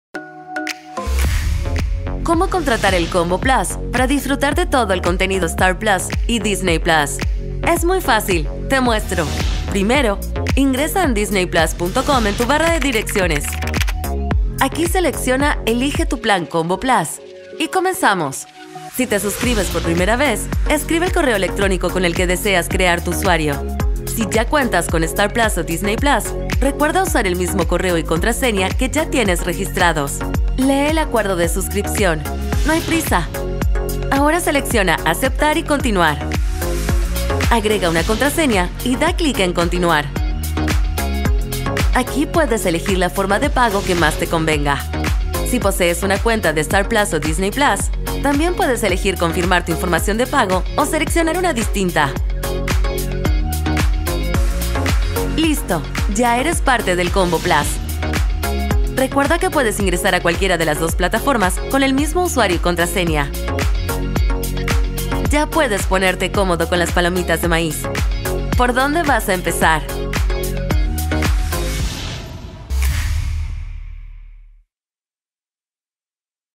¿Cómo suscribirse a Disney+ y Star+ en combo?
No te pierdas la oportunidad de disfrutar de los mejores contenidos de Disney y Star en un solo lugar.

► Estereofonica
Somos un medio independiente sobre música, televisión, cine y entretenimiento digital. Sigue más noticias y videos como este